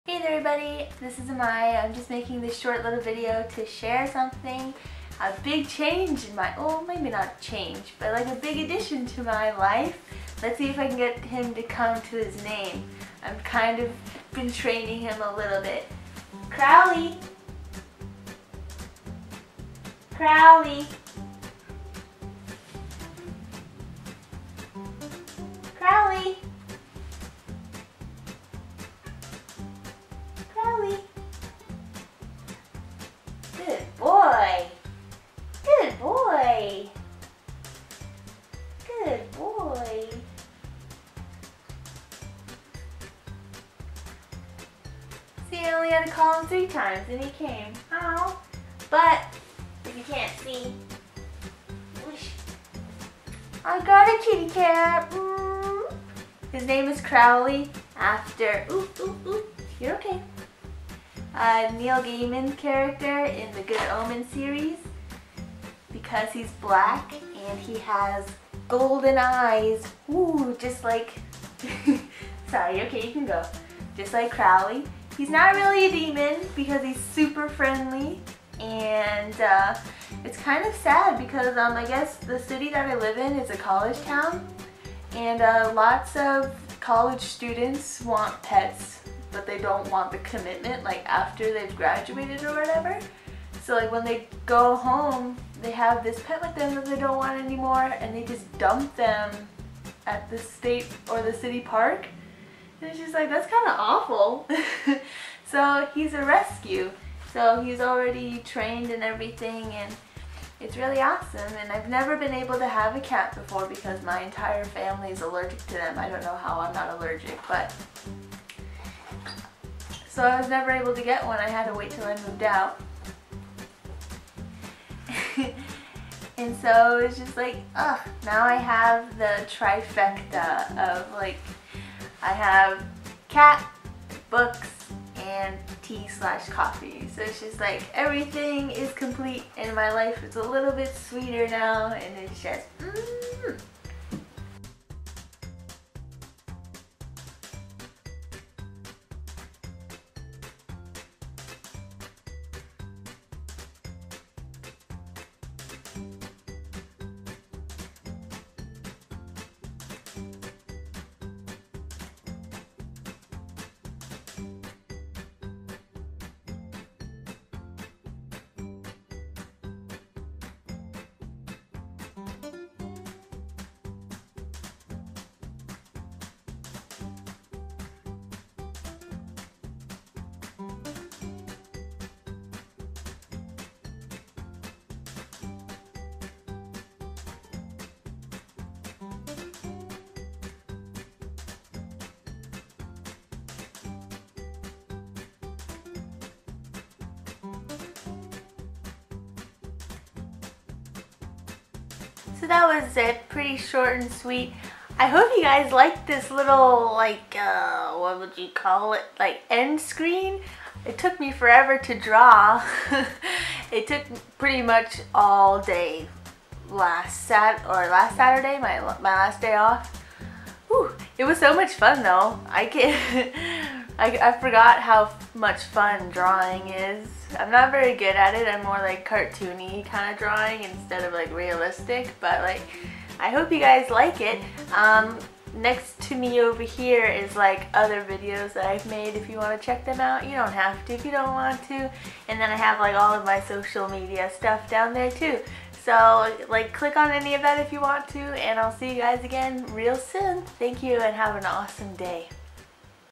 Welcome to my Home, Furry Friend!
Hello there everyone! I would like to announce the newest addition to the amai (。・∀・)ノ household! He has four positively adorable furry legs and mews when he wants food. His eyes are like liquid sunlight and he purrs incessantly. Welcome Crowley! I hope to have many delightful years with you! ❤❤